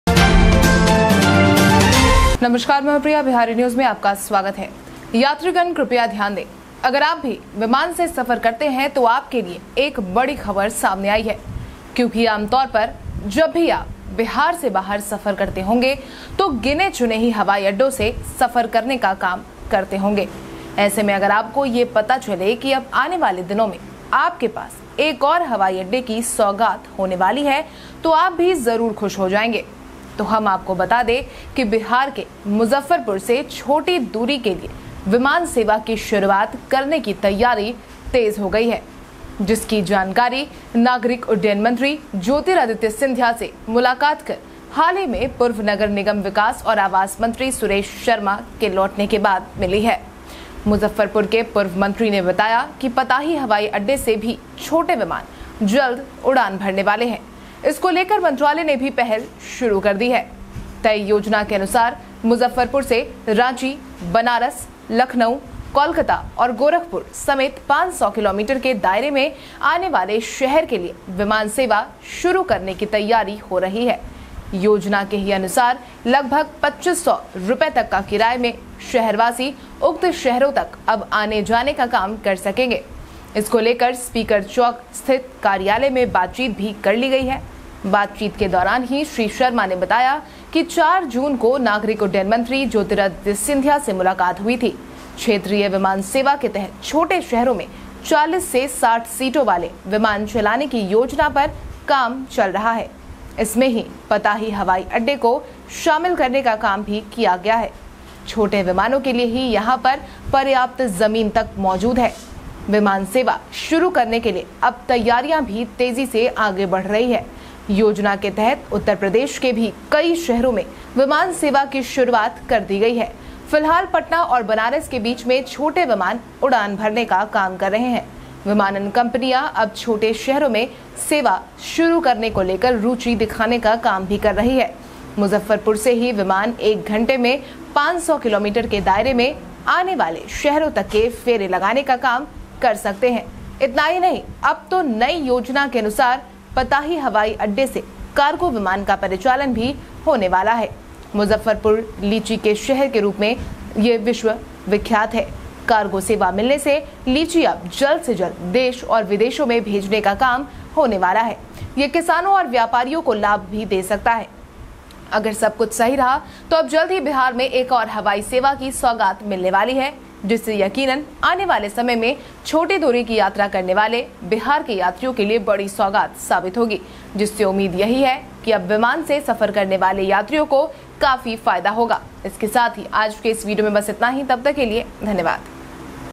हवाई यात्रियों को फायदा, बिहार में अब यहां से शुरू होगी हवाई सेवा
Travelers please note, if you also travel by plane, then a big news has come out for you, because generally whenever you are traveling outside Bihar, you must have done the work of traveling through few airports like this. If you come to know that now in the coming days you are going to have another airport, then you will also be happy, then let us tell you that the airline is about to start for a short distance from Muzaffarpur in Bihar. Preparations are being made about which, after meeting Civil Aviation Minister Jyotiraditya Scindia, recently former Municipal Development and Housing Minister Suresh Kumar Sharma has given this information,